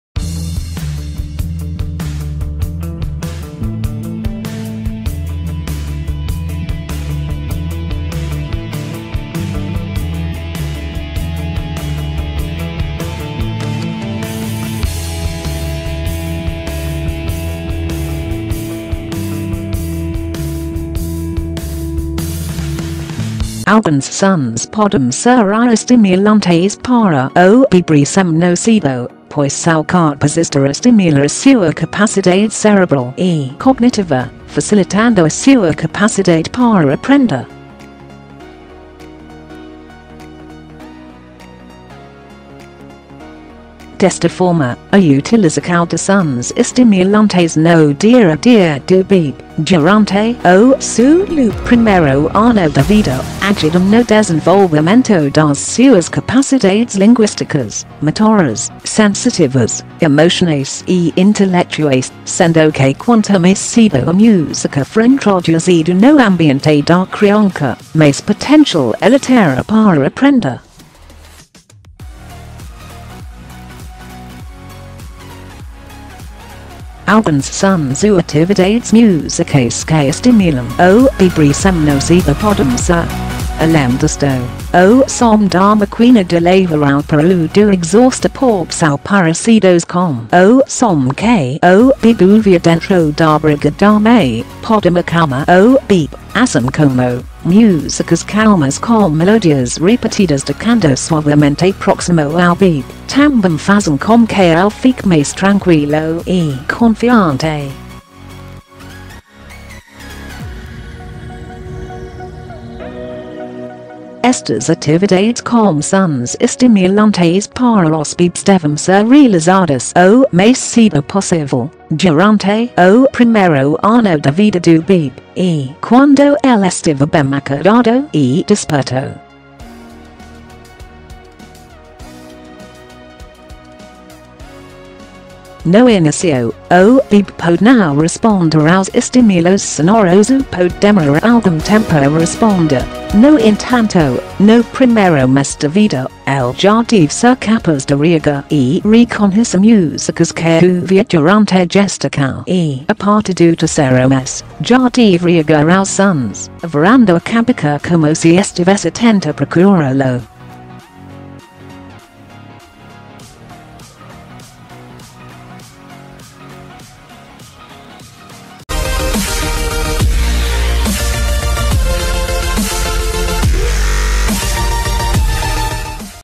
Saúde - Sons estimulantes para os bebês recém nascidos
Alguns sons podem ser estimulantes para o bebê recém-nascido, pois são capazes de estimular a sua capacidade cerebral e cognitiva, facilitando a sua capacidade para aprender.Desta forma, a utilização de sons estimulantes no dia a dia do bebê, durante o seu primeiro ano de vida, ajudam no desenvolvimento das suas capacidades linguísticas, motoras, sensitivas, emocionais e intelectuais, sendo que quanto mais cedo a música for introduzida no ambiente da criança, mais potencial ela terá para aprender.Alguns sons ou atividades musicais que estimulam o bebê recém nascido podem ser:  Além disto, o som da máquina de lavar roupa ou do exaustor porque são parecidos com o som que o bebê ouvia dentro da barriga da mãe, podem acalmar o bebê, assim como, músicas calmas com melodias repetidas tocando suavemente próximo ao bebê, também fazem com que ele fique mais tranquilo e confiante.Estas atividades com sons estimulantes para os bebês devem ser realizadas o mais cedo possível, durante o primeiro ano de vida do bebê, e quando ele estiver bem acordado e desperto.No início, o bebê pode não responder aos estímulos sonoros ou pode demorar algum tempo a responder, no entanto, no primeiro mês de vida, ele já deve ser capaz de reagir e reconhecer músicas que ouvia durante a gestação e a partir do terceiro mês, já deve reagir aos sons, virando a cabeça como se estivesse a tentar procurá-lo.